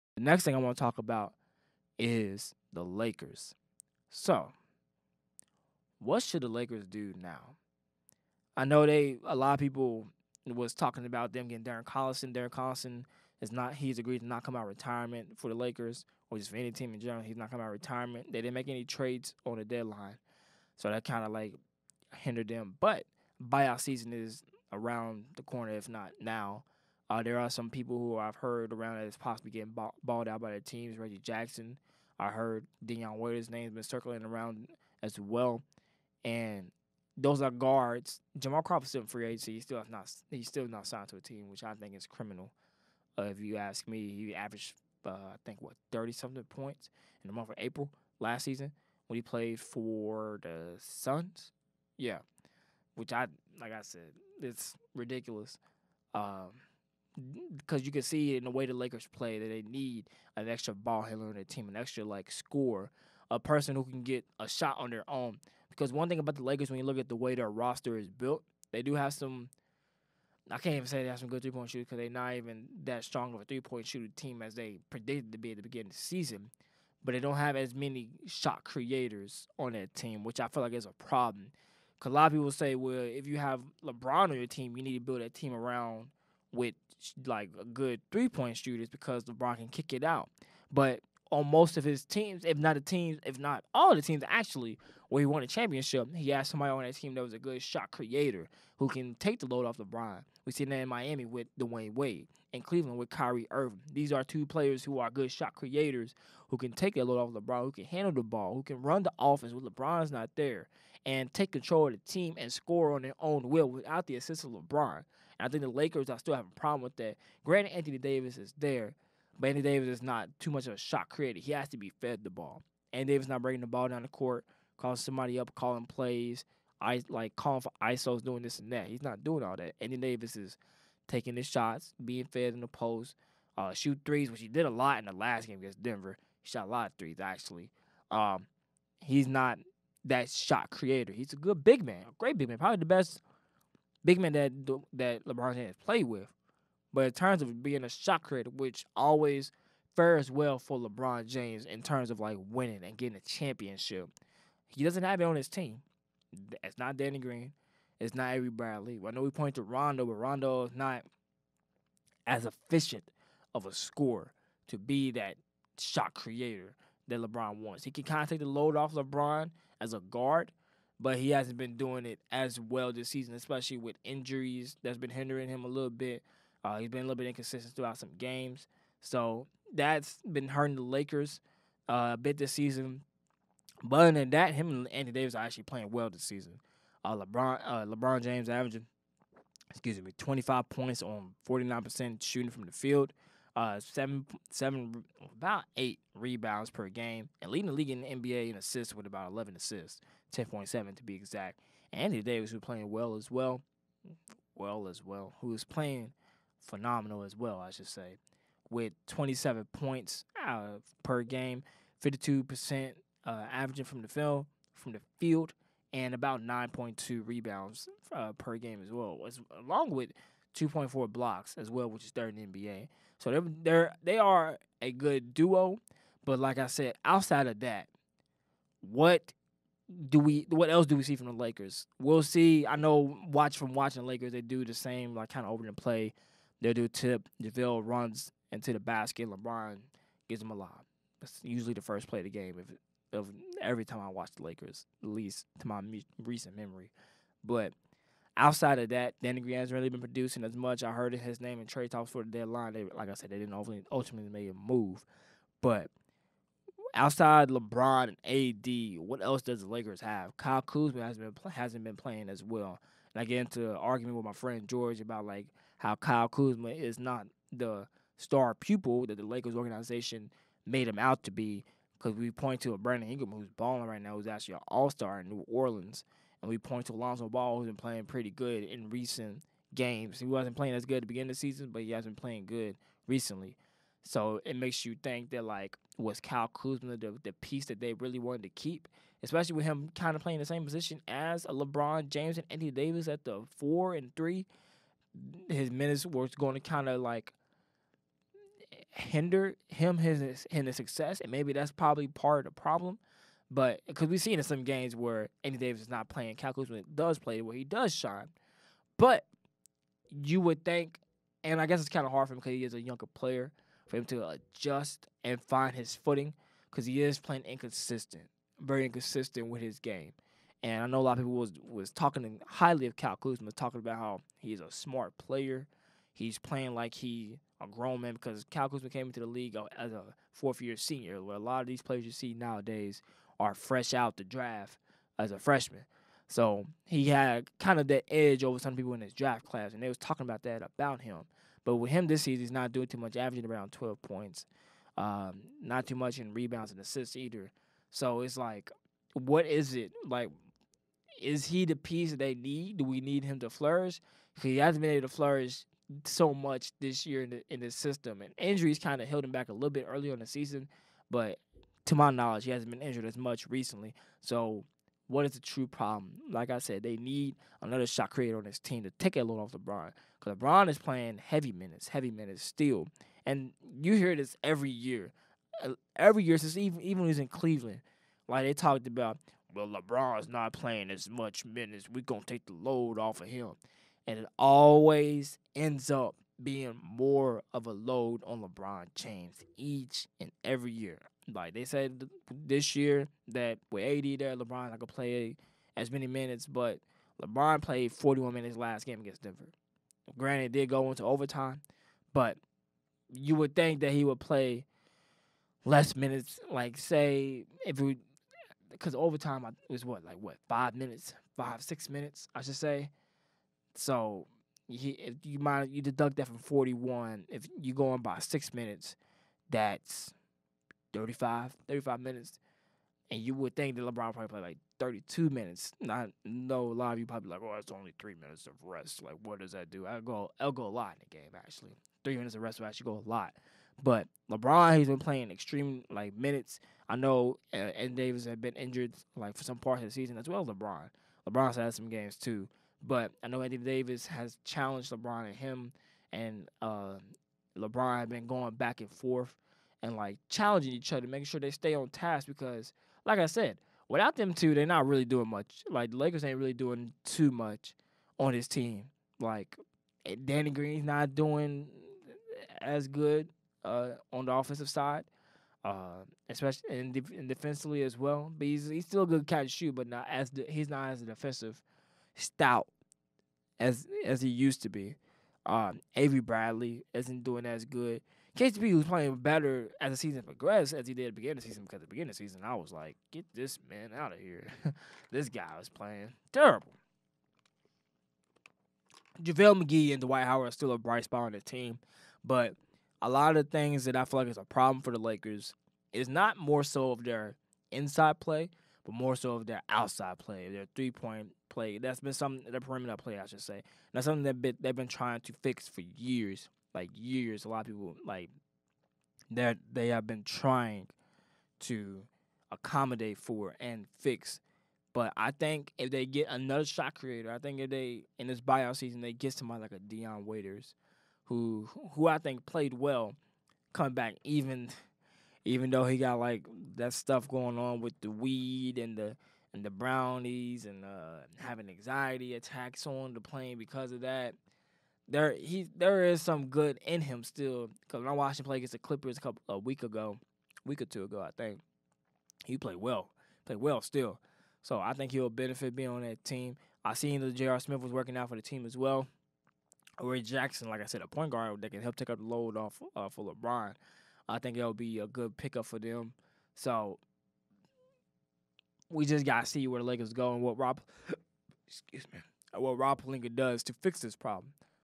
What should the Lakers do with Lebron after All-Star Break? - Tre Ball Episode 3
The Lakers are currently sitting on top of the western conference standings as the number 1 seed. This comes at the expense of Lebron carrying much of the load of the team. What should the Lakers to better their offense and get Lebron the help he deserves?